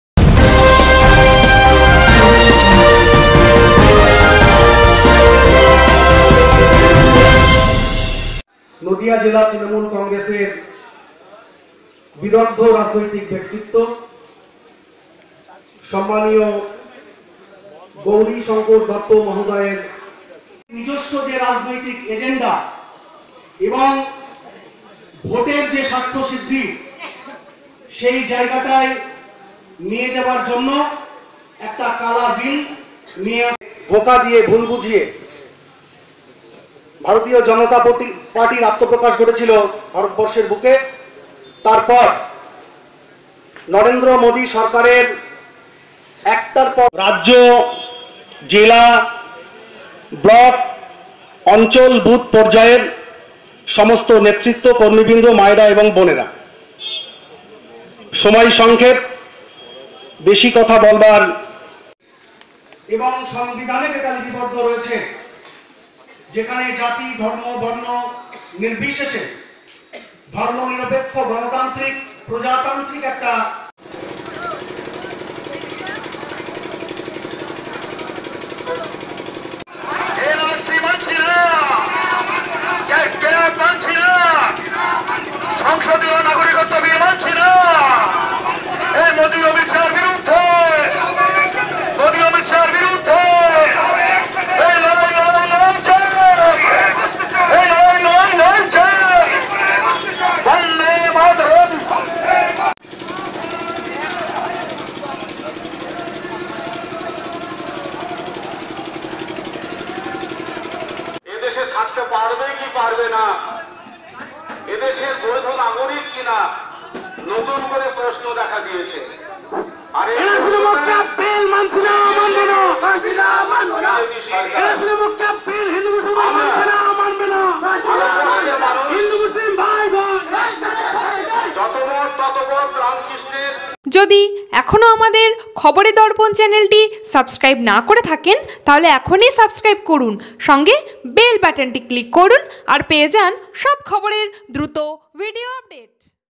নদিয়ার তেহট্টে বিধায়ক গৌরি শঙ্কর দত্তের নেতৃত্বে NRC ও CAB আইনের বিরুদ্ধে ধিক্কার সভা ও মিছিল
নদীয়ার তেহট্টে বিধায়ক গৌরি শঙ্কর দত্তের নেতৃত্বে তৃণমূল কংগ্রেস এনআরসি ও সিএবি এর প্রতিবাদে পথশোভার কর্মসুচী,এদিন বেলা তিনটে নাগাত শুরু হয় তেহট্ট বাজারে , মিটিং শেষে একটা মিছিল করে তেহট্ট বাজার থেকে হাউলিয়া পার্কমোড়ে পরিক্রমা করে।